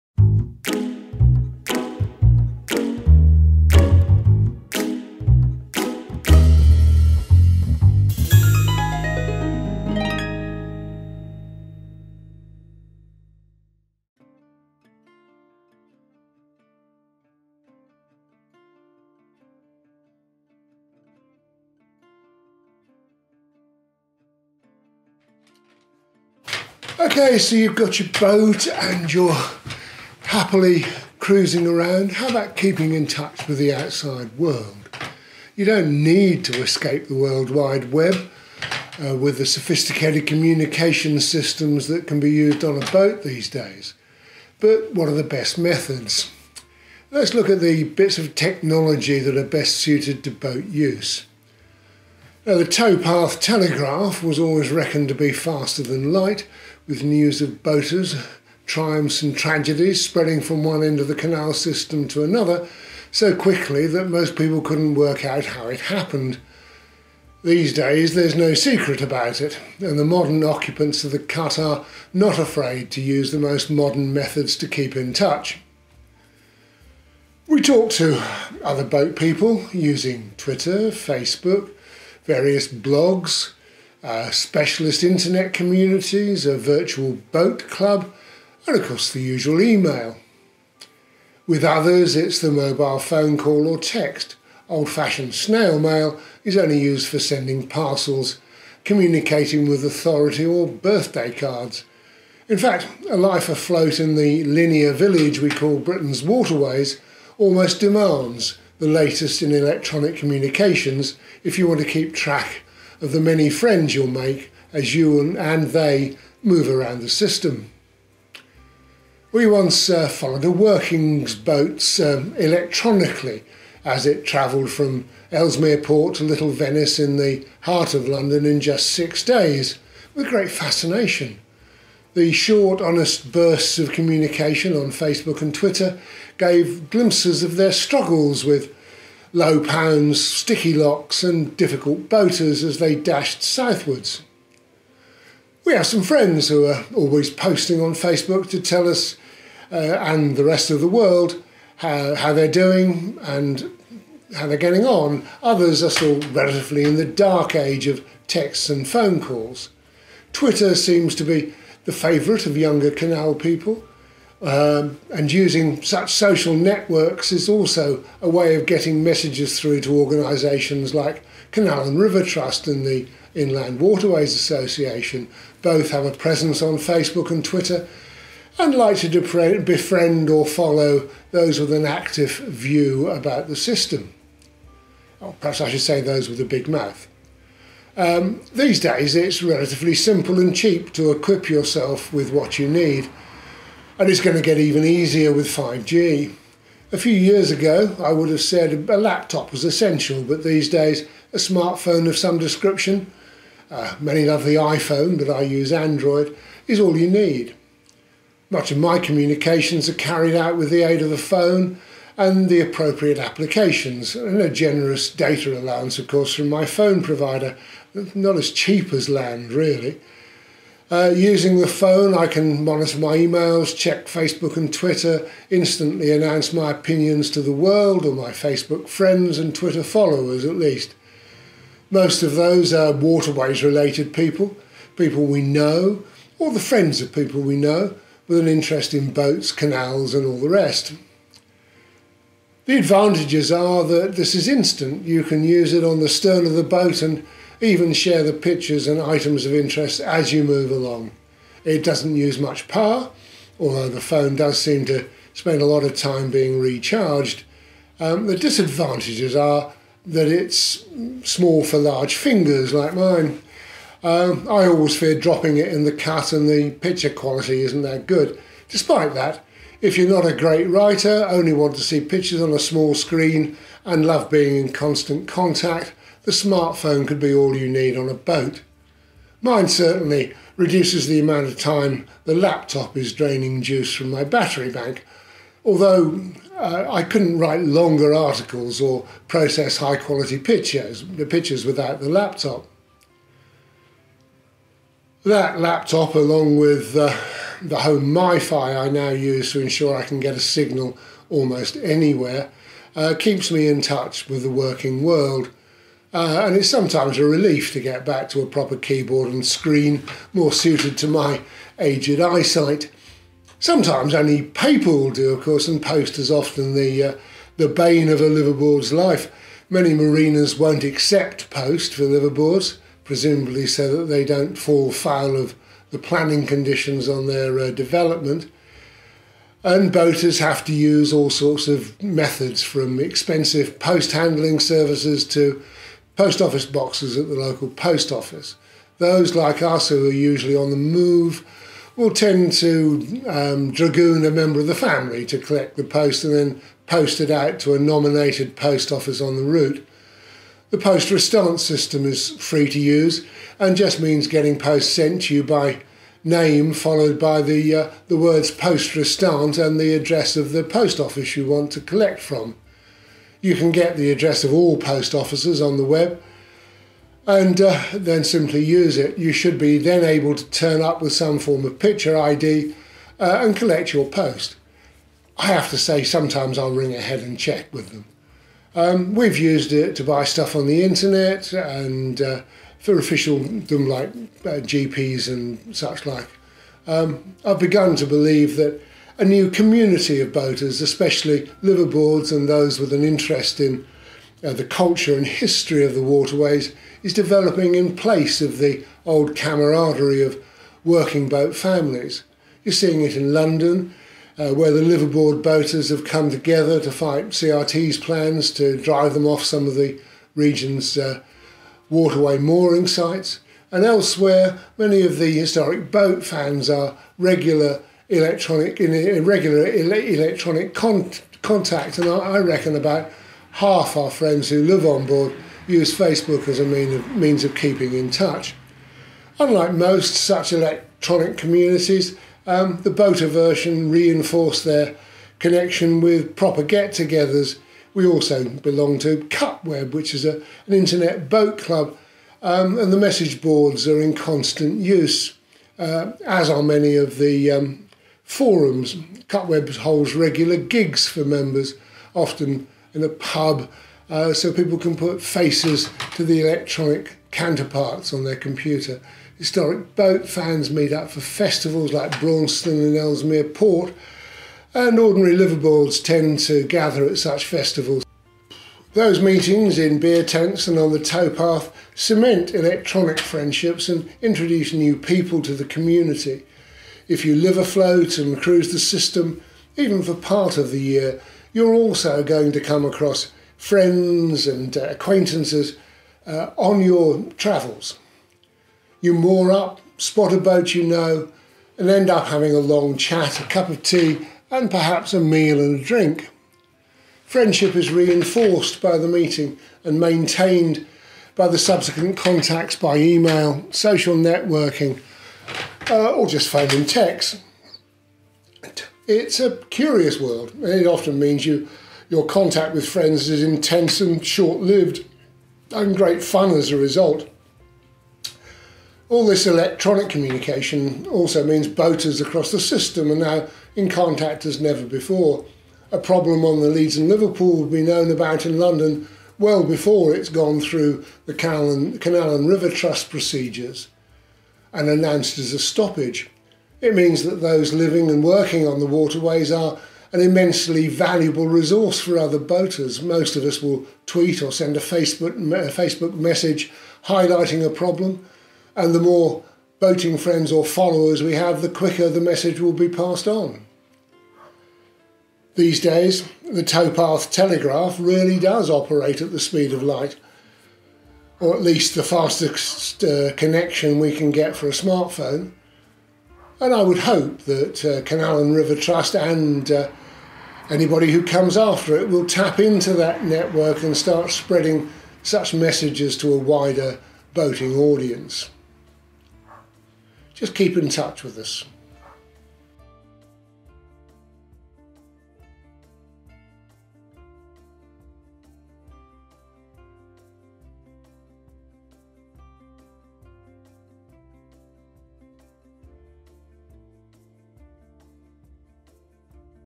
Norbury Knowhow Living Afloat Keeping in touch
Peter Underwood, long term boater, journalist and author of Living Afloat, looks at keeping in touch with the outside world whilst living aboard